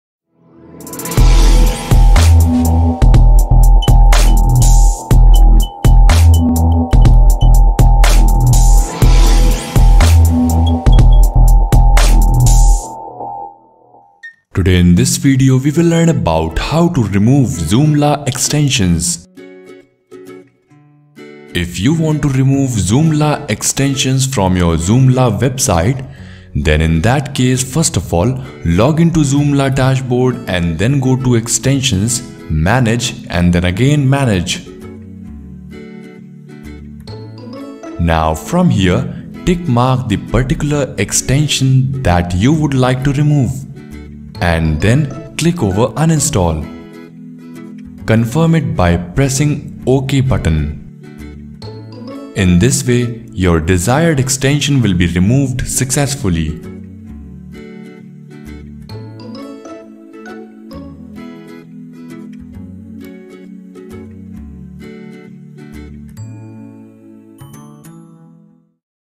How to Remove Joomla Extensions?
Today in this video we will learn how to remove Joomla extensions.

Step::1 Login to your Joomla dashboard
Step::2 Go to Extensions tab and click on Manage section and again click on Manage
Step::3 Select the extension which you want to remove and click on Uninstall icon at the top
Step::4 Confirm the uninstallation process by clicking on Yes button.
In this way your desired extension will be removed from your Joomla website.